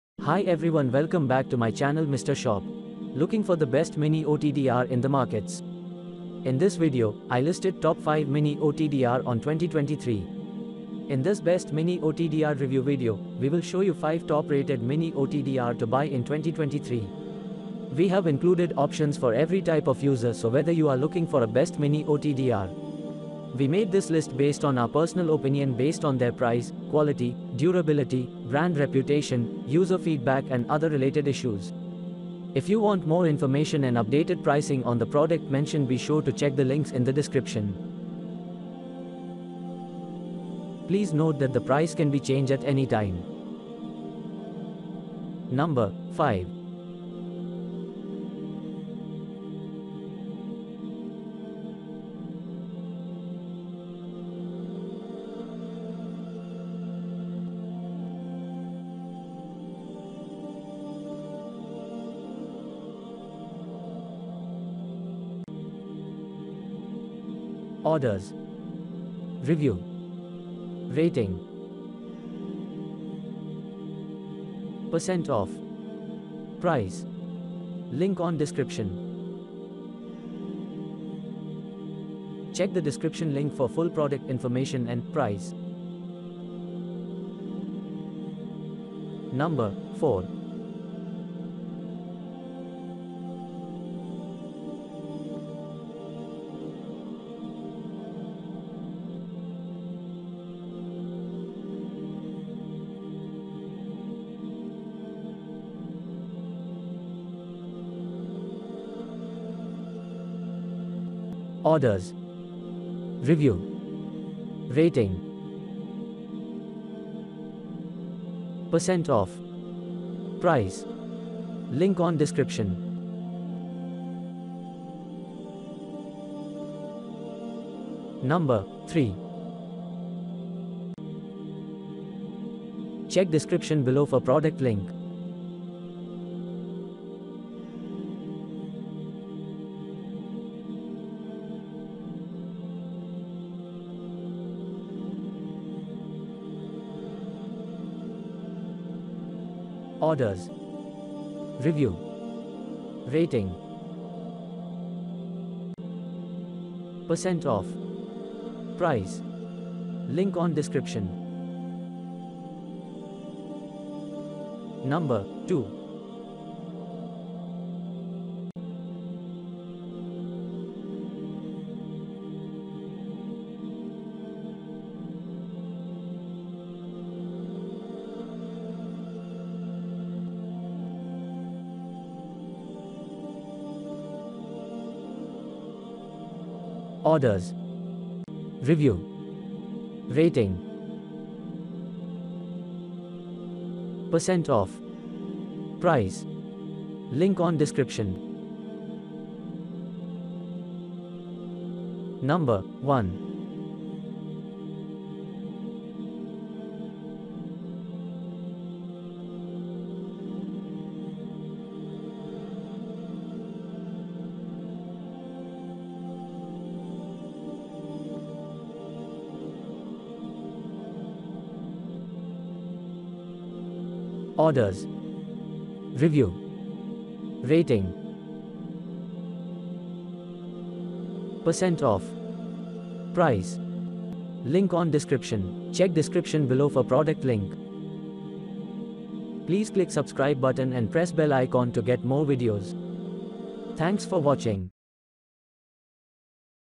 Top 5 Best Mini OTDR In 2024 | Fiber Optic Reflectometer
05.OFW Mini OTDR iLOM Optical Time Domain Reflectometer

04. Pro Mini OTDR active fibe Fiber Optic Reflectometer

03. OFW Mini OTDR Optic Otdr Optical Reflector Active Fiber Live Tester

02. Mini OTDR Active Fiber Live Test 1550nm 20dB Optical Fiber Reflectometer

01. Pro Mini OTDR Fiber Optic Reflectometer 980rev